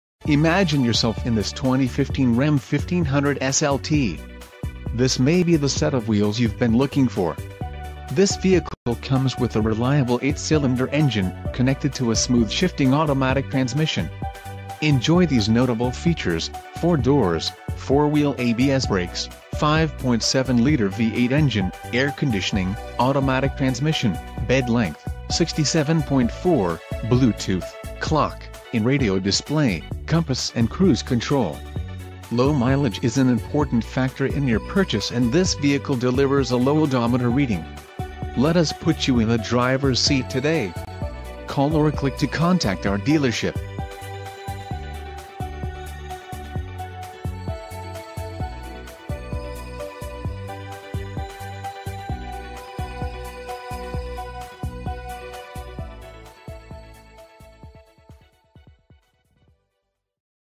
2015 RAM 1500 SLT - Benny Boyd CDJ - Lampasas, TX 76550
2015 RAM 1500 SLT Condition: Used Mileage: 7125 Transmission: Automatic 8-Speed Exterior Color: White Stock